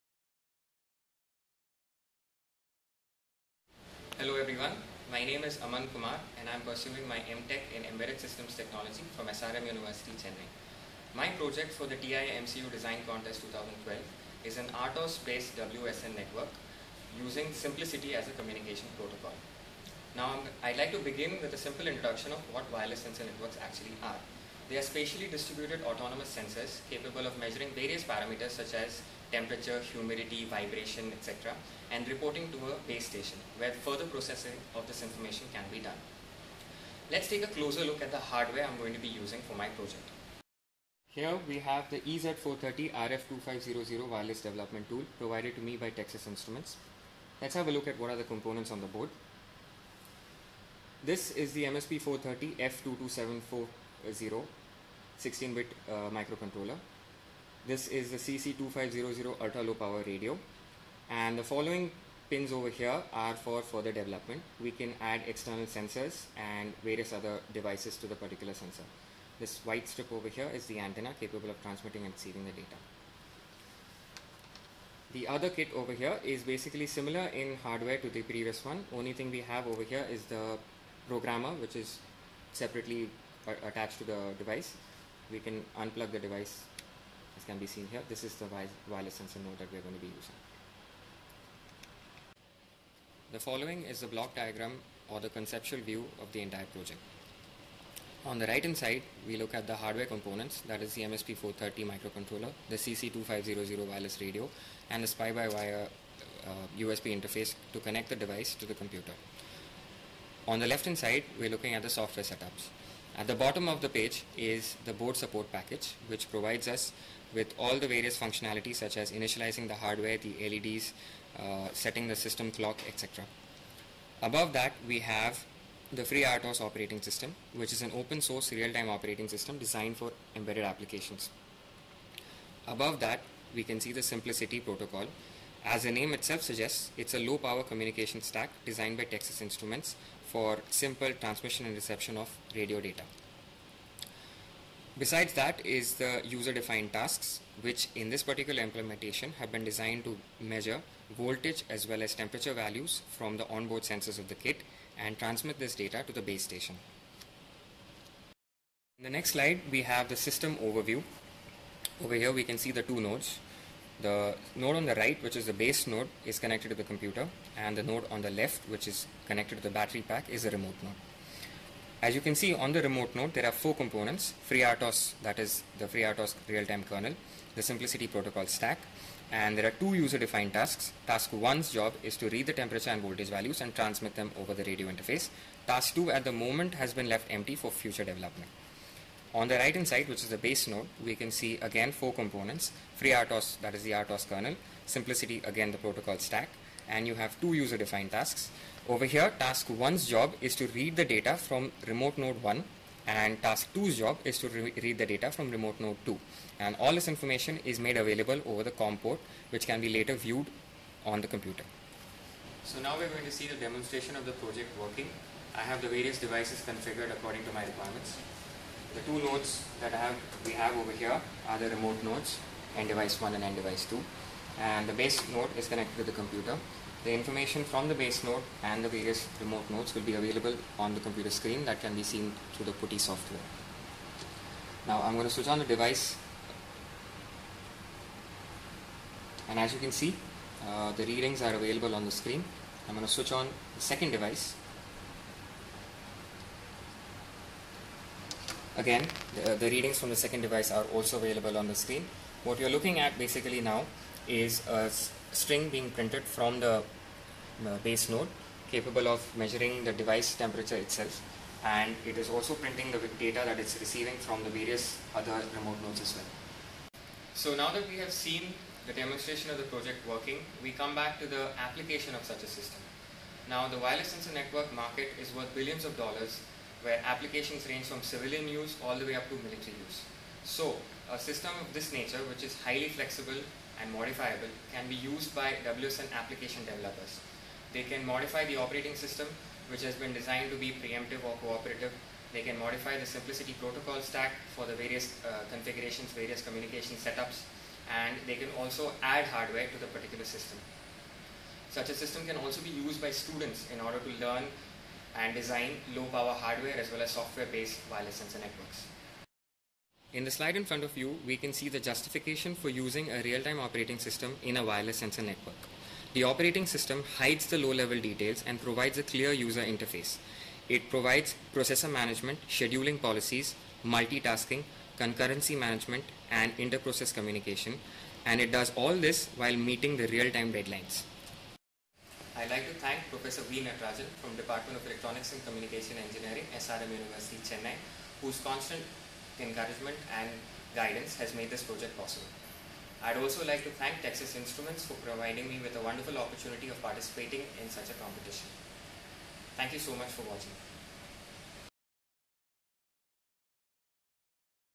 Texas Instruments India TI MCU Design Contest 2012 RTOS based WSN Node with SimpliciTI.wmv
Unique Combination of FreeRTOS Real time Operating System,  TI MSP430 F2274 microcontroller and SimpliciTI wireless communication protocol in a Wireless Sensor Network.